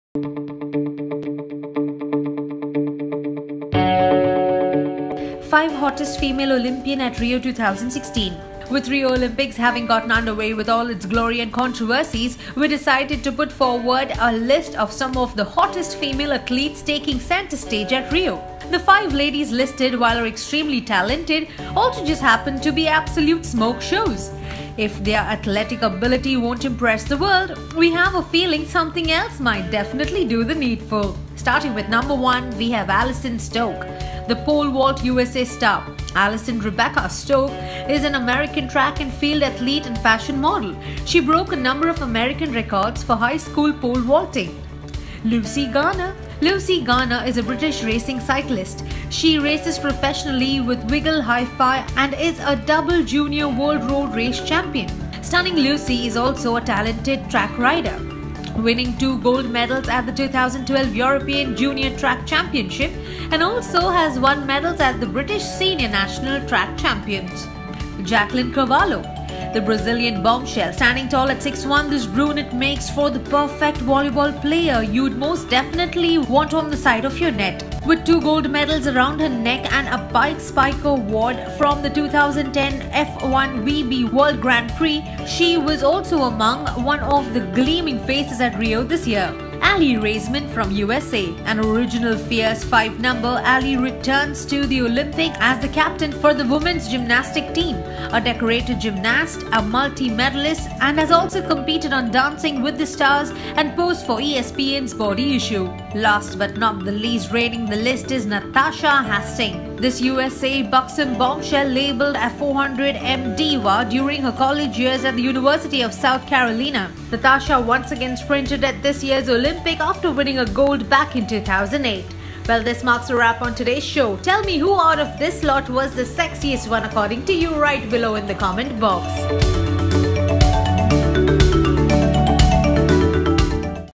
Top 5 Most Beautiful Female Athletes At The Olympics | Last Page Readers
With Rio Olympics got underway with all its glory and controversies, we decided to put together a list of some of the hottest female athletes taking center stage in Rio. If their athletic ability won't impress the world, we have a feeling something else might. Watch this video Now!!